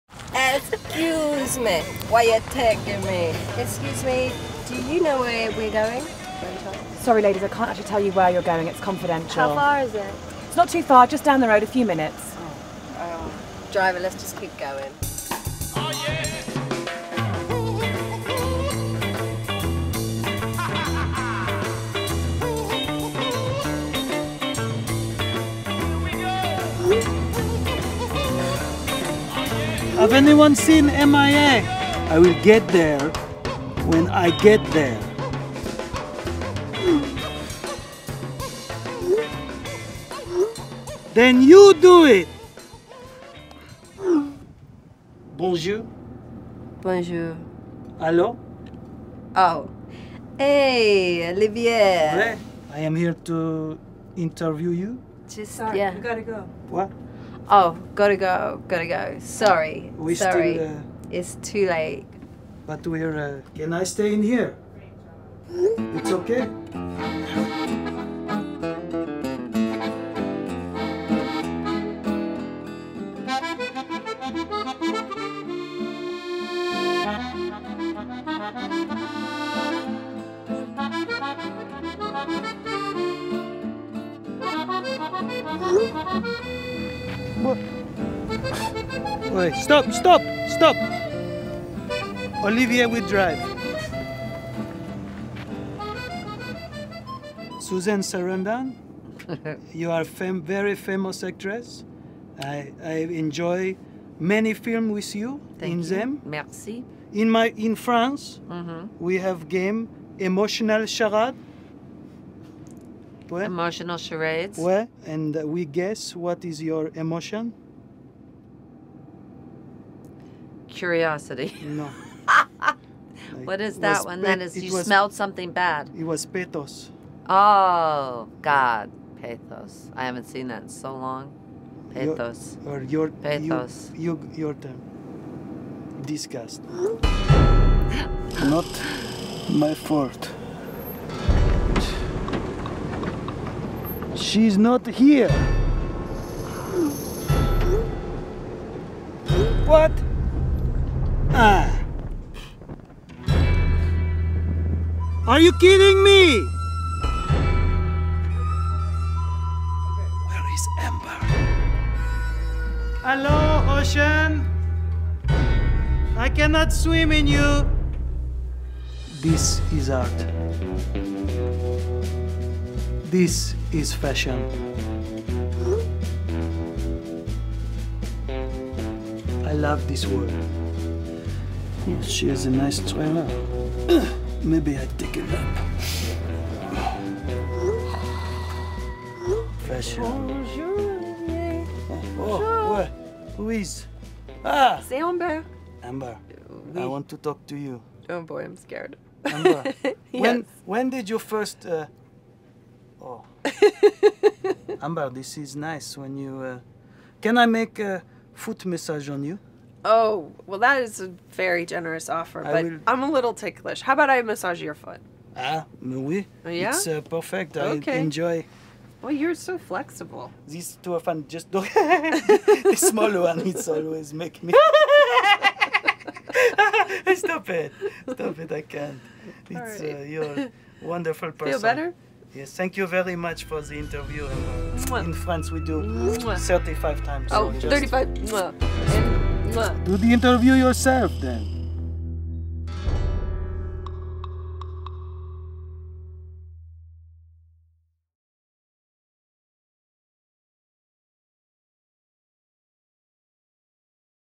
Purple x Mercedes-Benz Fashion: Petit Oz as Olivier Zahm | 2017 Fashion Story | Concept EQ
Join Olivier Zahm’s puppet assistant Petit Oz behind the scenes at the Mercedes-Benz 2017 Fashion Story mbcollective on location in Malibu, California, where he interviews the stars of the 2017 campaign: M.I.A., Susan Sarandon and Amber Valletta.

Titled “Generation Now, Generation Next”, each chapter of the mbcollective Fashion Story for 2017 will unite exciting international talents in the form of a protagonist (Generation Now), their accompanying protégées (Generation Next) and the new generation battery-electric Concept EQ.